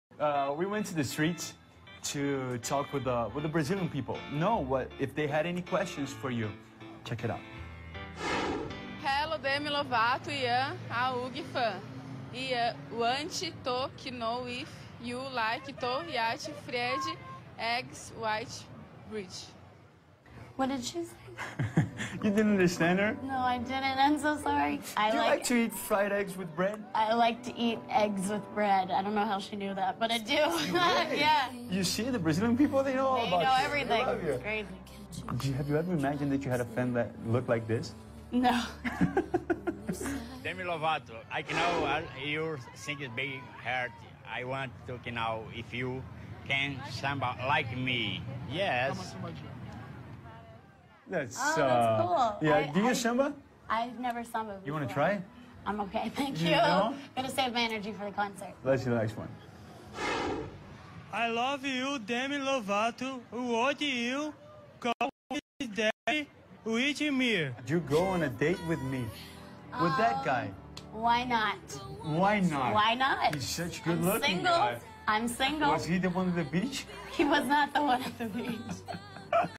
Demi Lovato: She Likes To Eat Eggs with Bread / She's Totally Single
[April 2012] During her tour in Brazil, Demi answered  a lot of personal questions.
It's kinda weird, but pretty funny. Poor Demi!:)